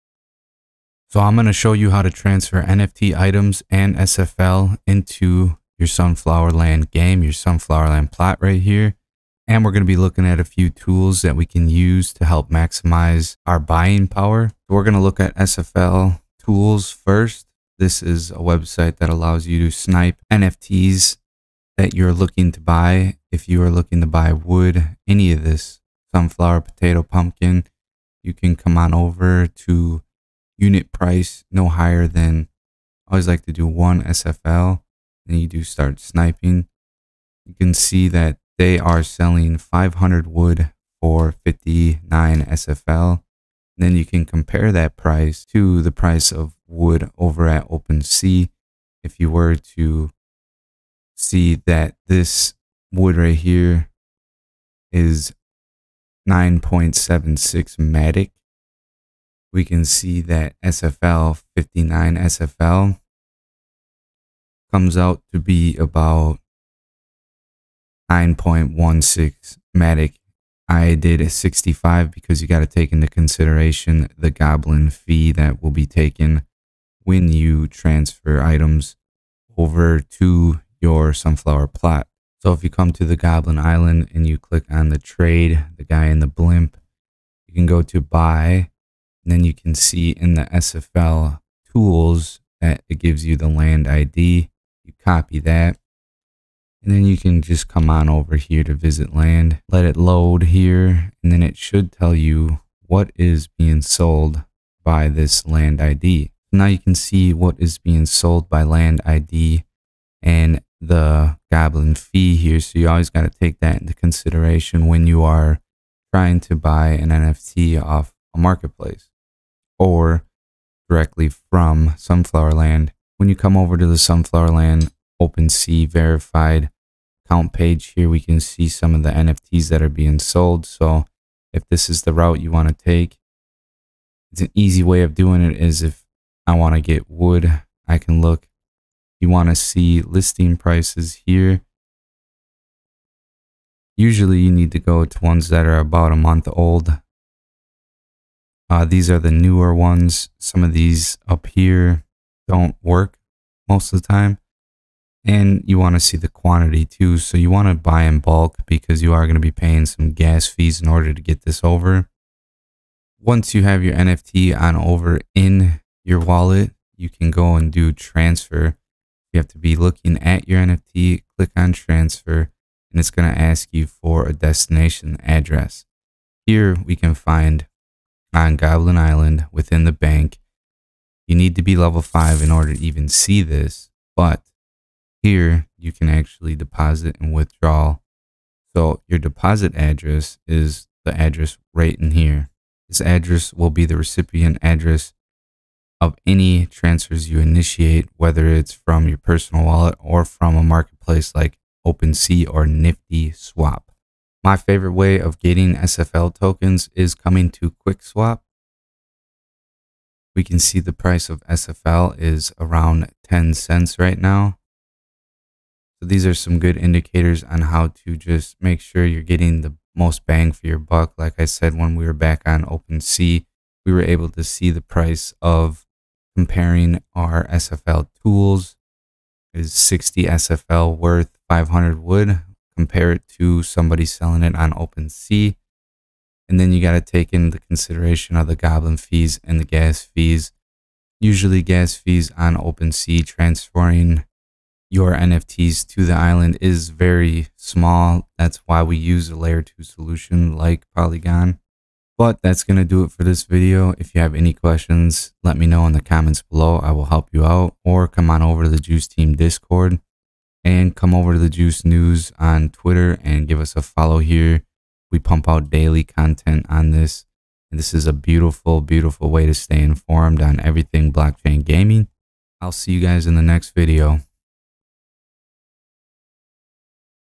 How to Transfers SFL/NFT Sunflower Land Sell/Buy NFT
This is a guide on how to transfer your NFT's from Opensea to sunflower land Looking to speed up your progress on SFL? I will show you how to Buy NFT's from OpenSea and how to use SFL Tools. SFL Tools is amazing! Its Live Listings See Sunflower Land listings in real-time! New listings appear immediately and flash green. Purchased listings flash red then disappear. Cancelled listings flash orange then disappear.

                                My Goal is to Educate
Thanks for all your Love and Support for the Team!

                          TIMESTAMPS:
0:00 Intro
0:14 How to use SFL.Tools
2:42 How to Send SFL/NFT's to Sunflower land wallet
3:23 How to get SFL
4:11 Outro